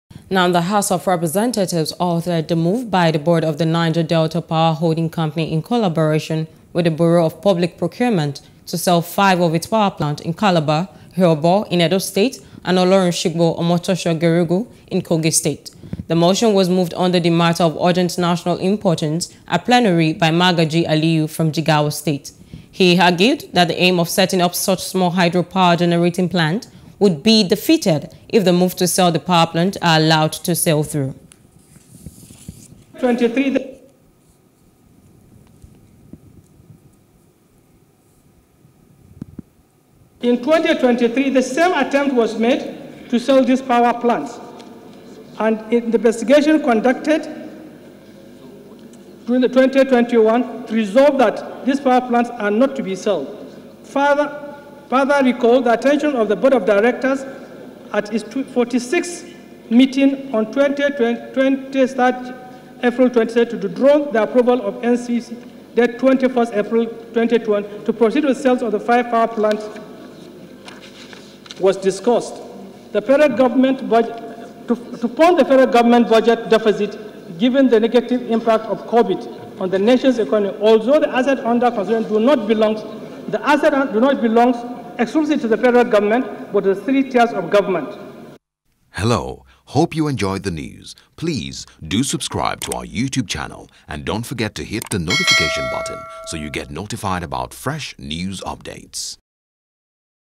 Energy: Reps Vote To Stop Privatization Of 5 Power Plants Of NDPHC | NEWS
The house of representatives halted the move by the Board of the Niger Delta Power Holding Company in collaboration with the Bureau of Public Procurement to sell five of its power plant in Calabar, Ihorbor in Edo State and Olorunsigbo, Omotosho, Geregu in Kogi State.